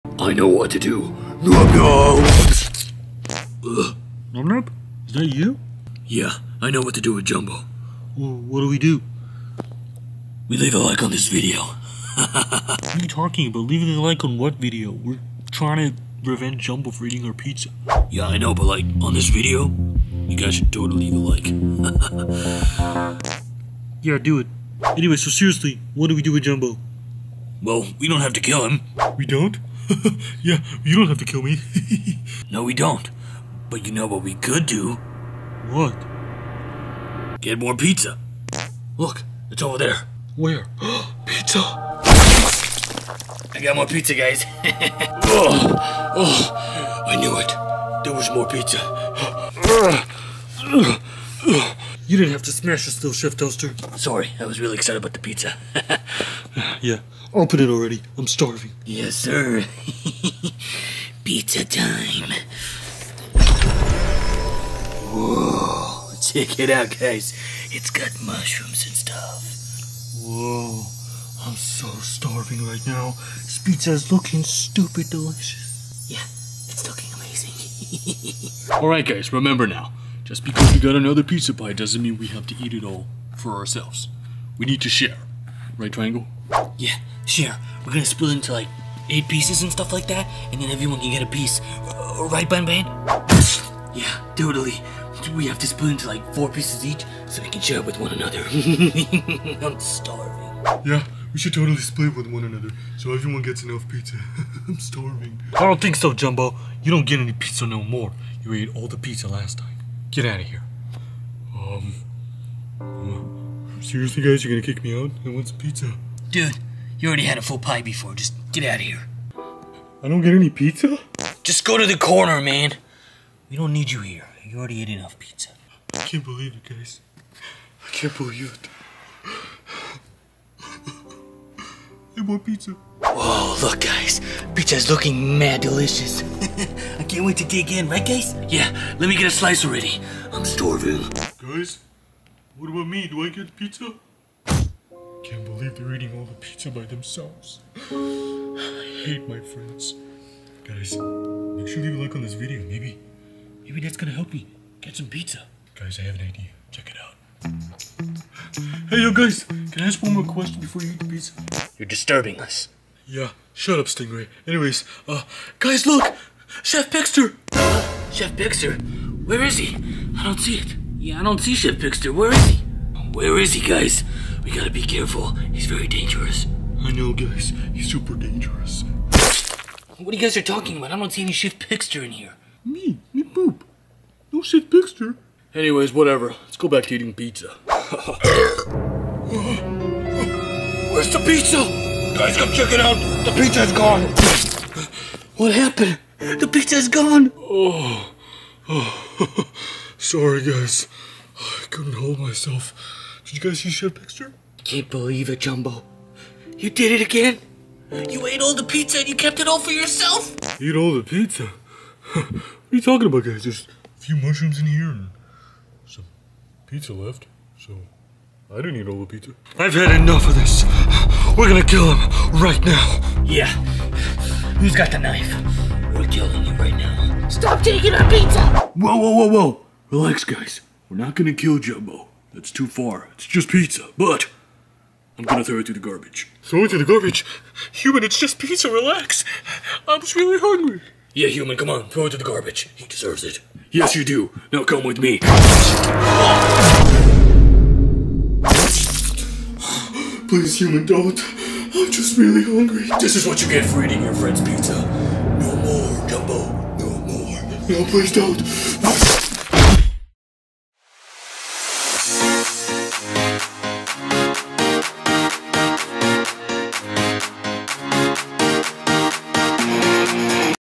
Garten of Banban (Pizza Time) Part 2
Garten of Banban Plushies - Pizza Time

It’s pizza time! The shapes and fatten of banban gang are having pizza for lunch! Will they all share the pizza and get along? Let’s see.